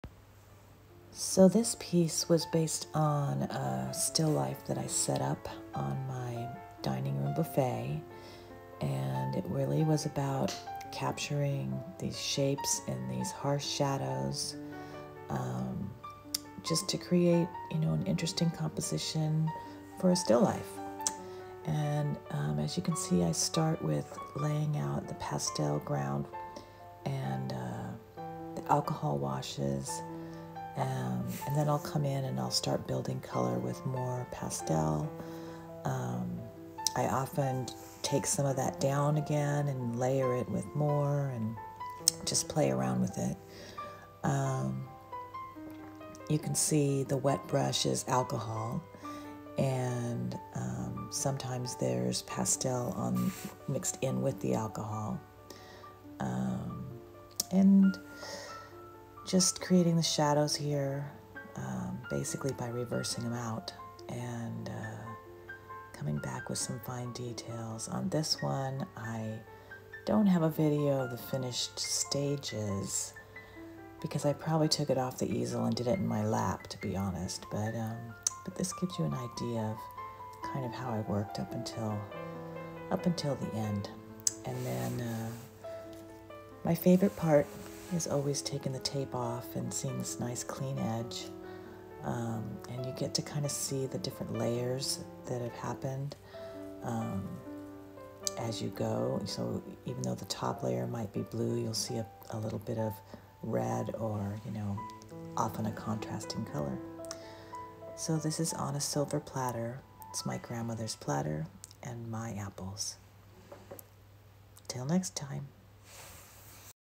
On a Silver Platter | Soft Pastel Demo
A little narration on some of my process for building a soft pastel painting.
Apple still life time lapse video.